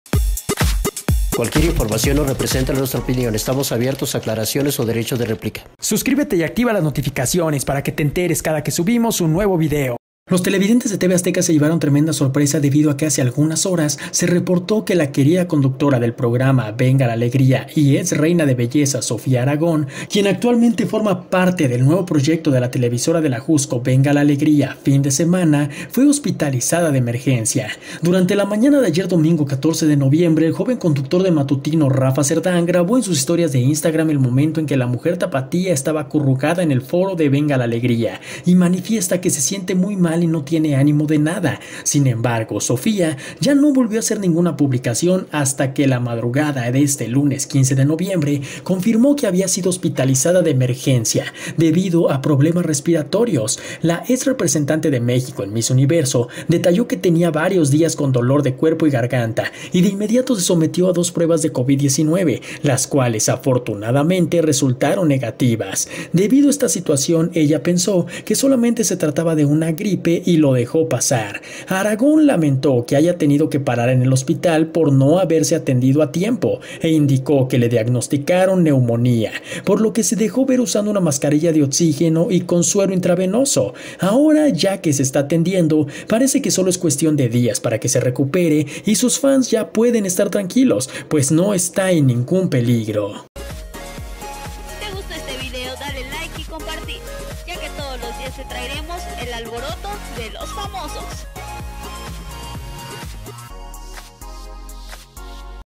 Conductora de Venga la Alegría, Sofia Aragón es hospitaIizada
Sigue diariamente las mejores noticias del medio del espectáculo, solo aquí en el Alboroto de los Famosos.